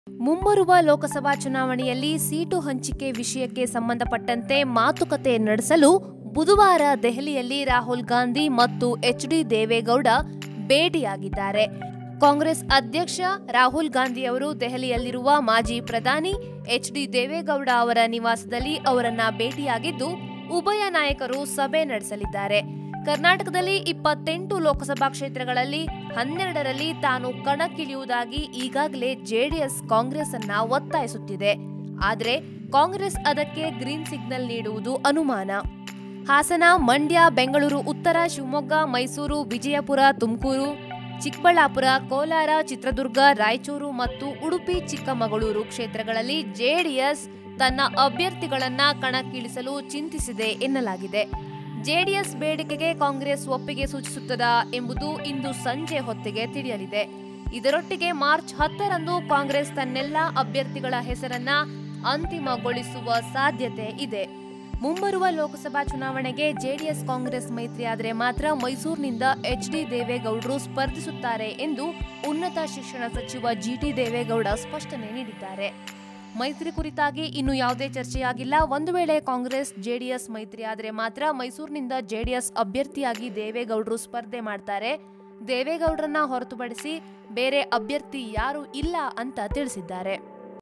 Lok Sabha Elections 2019: ಲೋಕಸಭೆ ಸೀಟು ಹಂಚಿಕೆ: ದೆಹಲಿಯಲ್ಲಿ ದೇವೇಗೌಡ-ರಾಹುಲ್ ಗಾಂಧಿ ಭೇಟಿ |Oneindia Kannada
lok sabha elections 2019: ಮುಂಬರುವ ಲೋಕಸಭಾ ಚುನಾವಣೆಯಲ್ಲಿ ಸೀಟು ಹಂಚಿಕೆ ವಿಷಯಕ್ಕೆ ಸಂಬಂಧಿಸಿದಂತೆ ಮಾತುಕತೆ ನಡೆಸಲು ಬುಧವಾರ ದೆಹಲಿಯಲ್ಲಿ ರಾಹುಲ್ ಗಾಂಧಿ ಮತ್ತು ಎಚ್ ಡಿ ದೇವೇಗೌಡ ಭೇಟಿಯಾಗಿದ್ದಾರೆ.

 Congress President Rahul Gandhi meets former Prime Minister and JDS leader HD Deve Gowda in his Delhi resedence, to hold discussions on the issue of seat-sharing in Karnataka for the coming Lok Sabha Elections,
Oneindia kannada